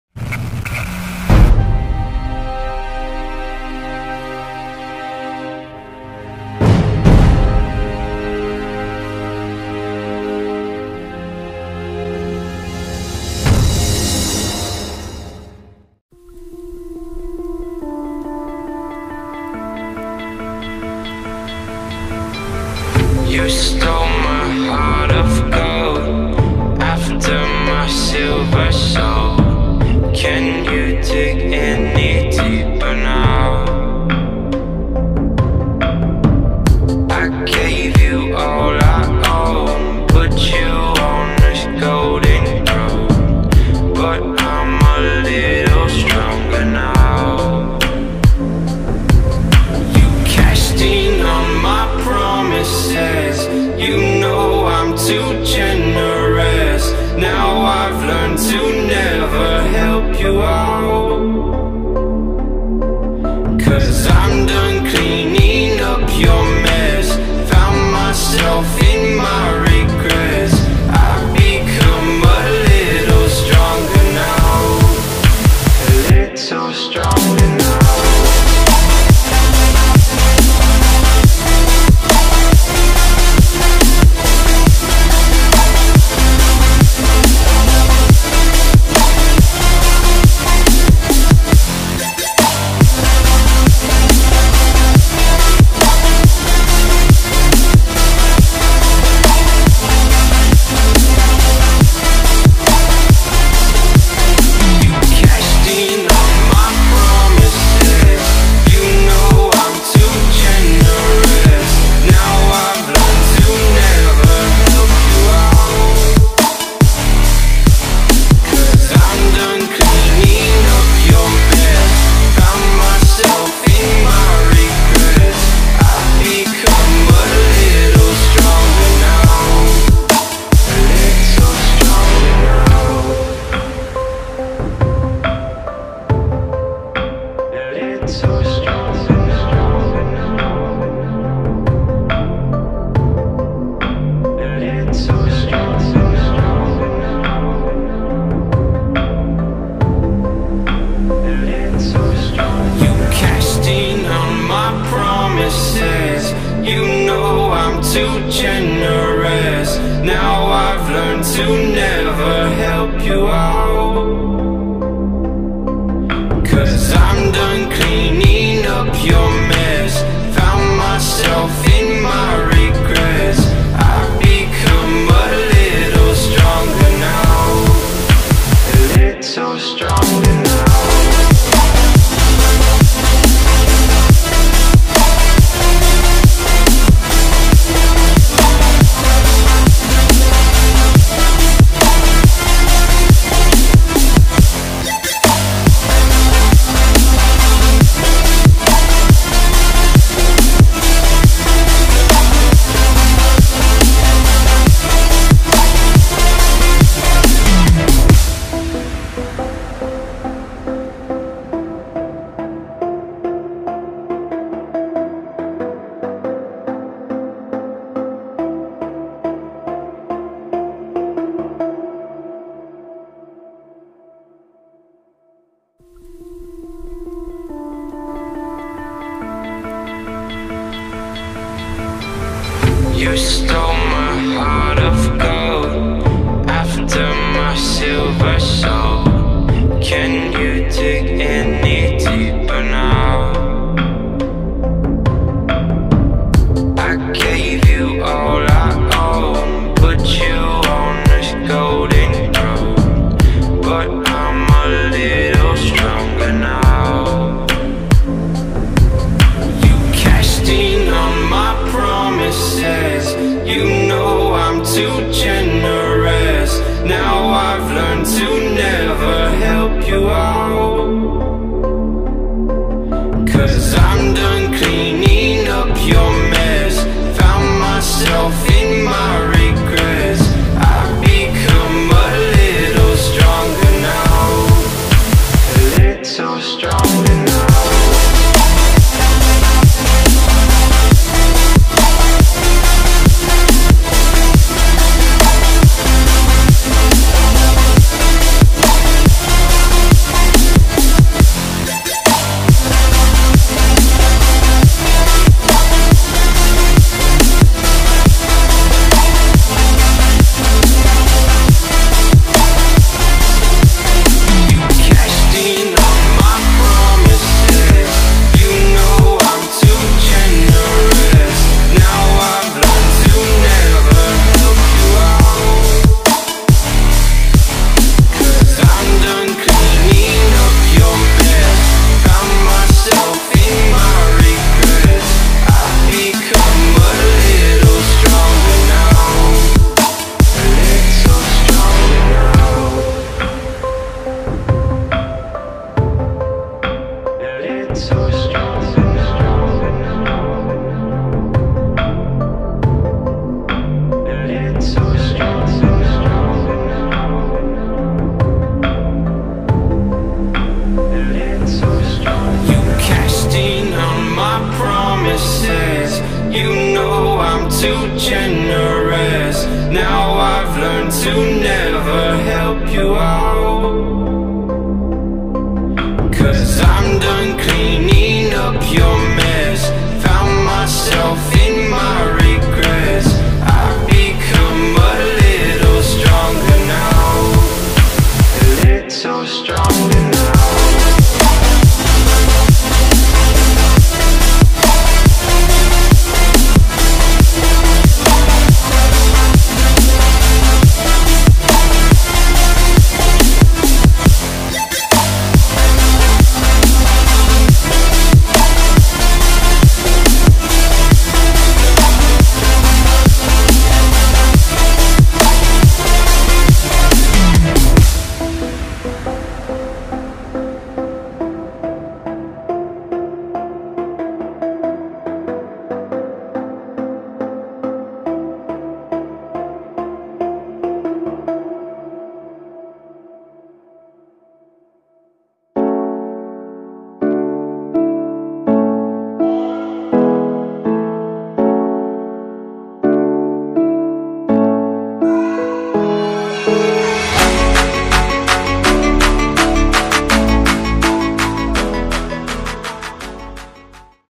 BEGINILAH PABRIK PROSES PEMBUATAN EXCAVATOR LIEBHERR DARI NOL SAMPAI JADI
Terimakasih